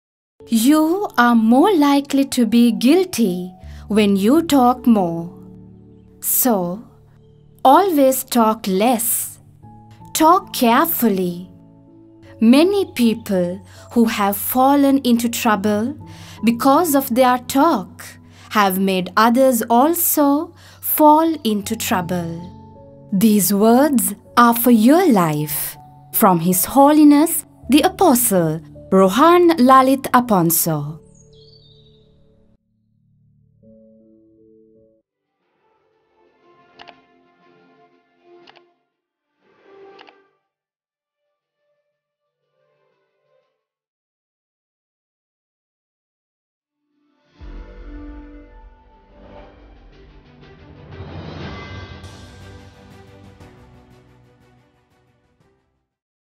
Always talk less, talk carefully | Quotes | Life Lessons
You are more likely to be guilty when you talk more. So, always talk less, talk carefully. Many people, who have fallen into trouble because of their talk have made others also fall into trouble

* These are the most powerful quotes and lessons to bring out your inner strength from within. *